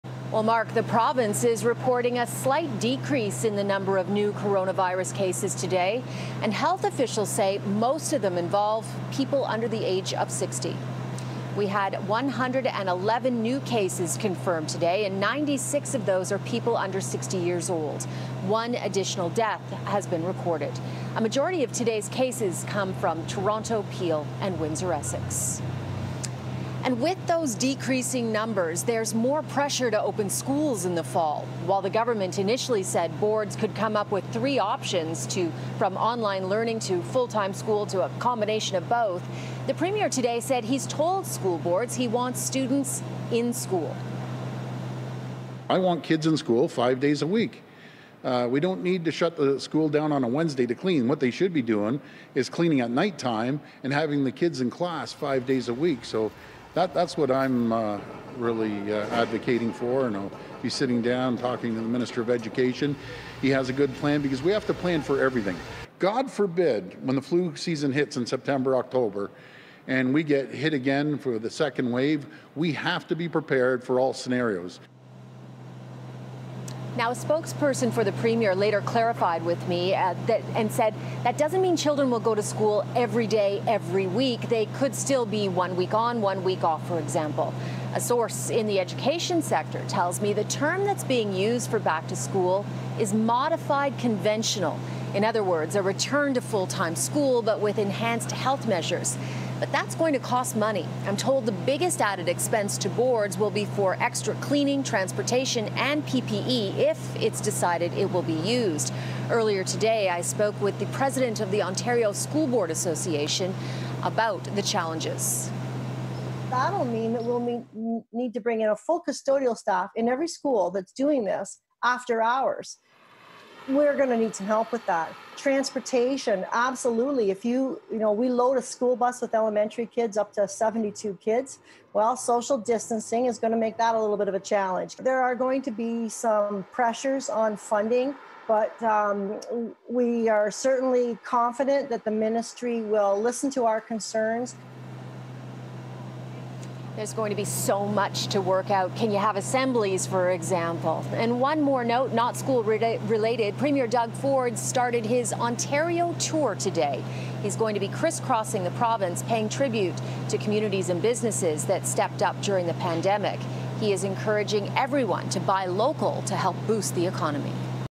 Mounting pressure to reopen schools in the fall
The premier says he wants children in class five days a week when school returns. Cynthia Mulligan reports on what that means for school children and the new term.